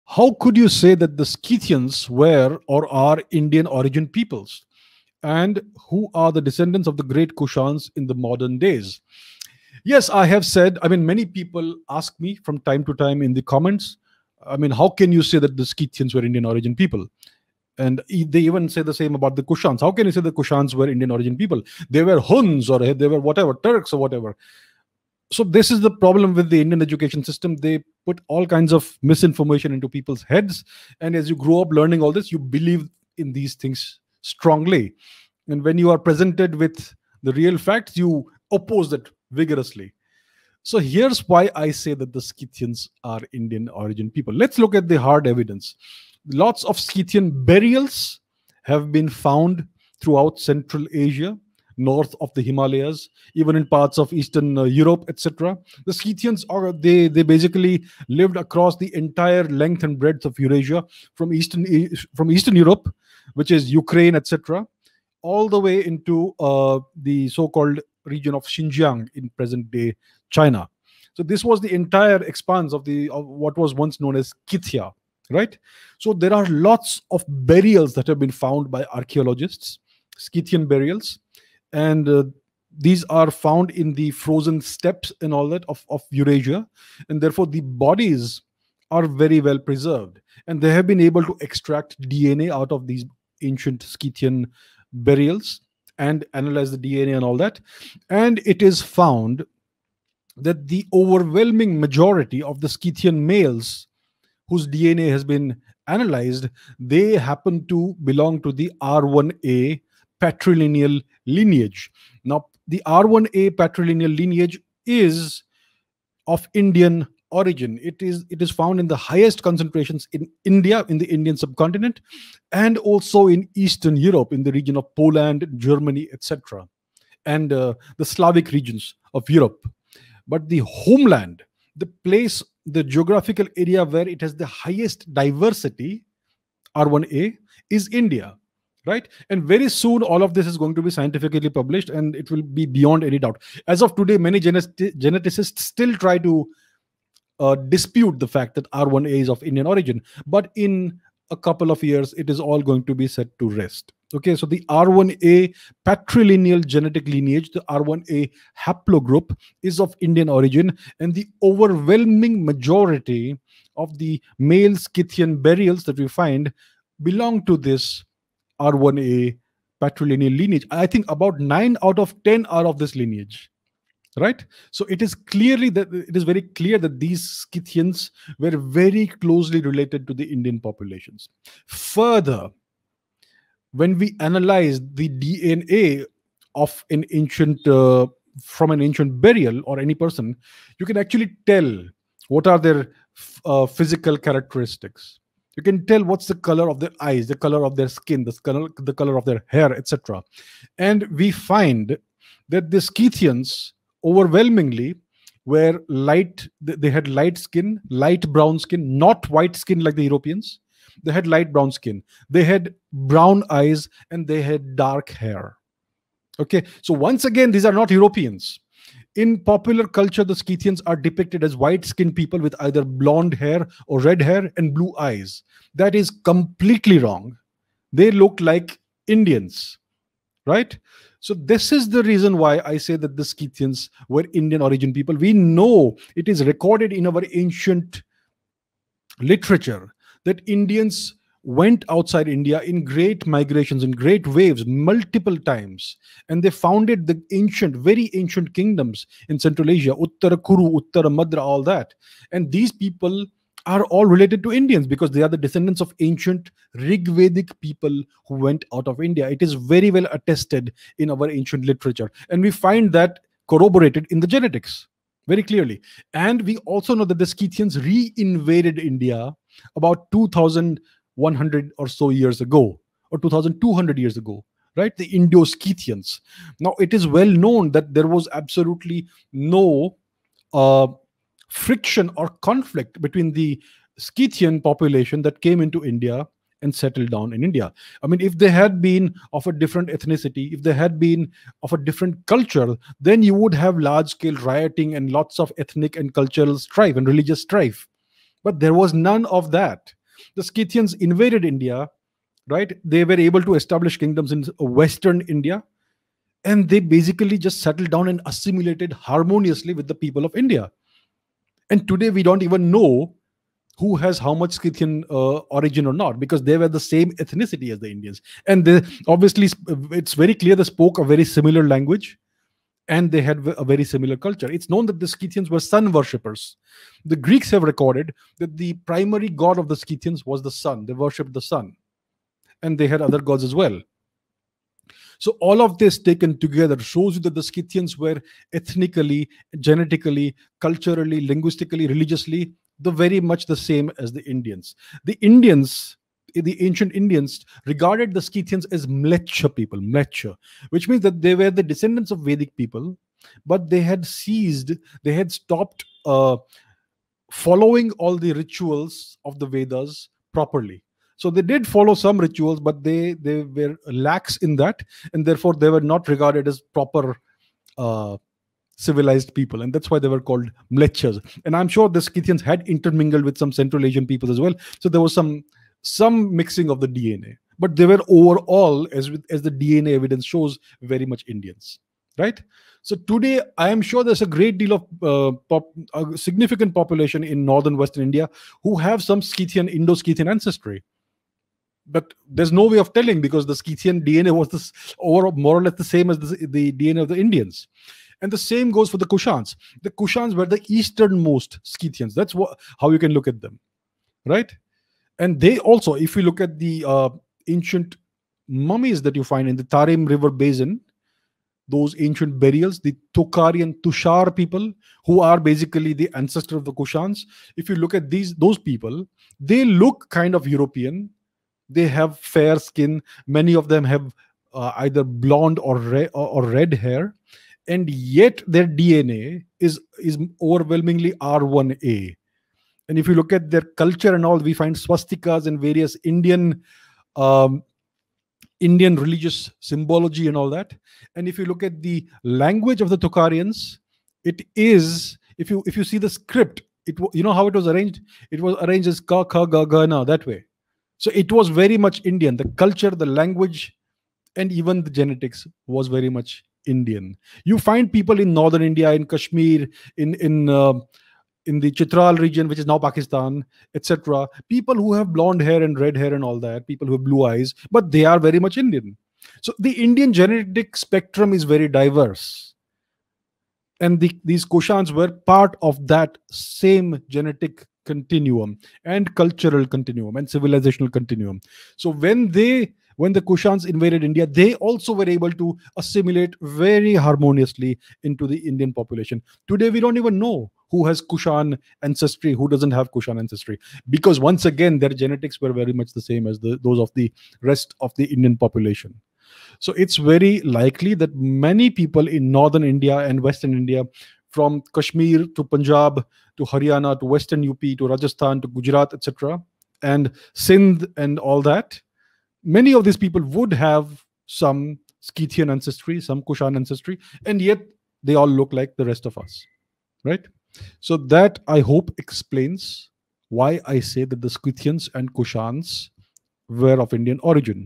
Did Scythians & Kushans Have Indian Ancestry? | AskAbhijit E35Q5 | Abhijit Chavda
Did the Scythians and Kushans have Indian ancestry? Who are their descendants?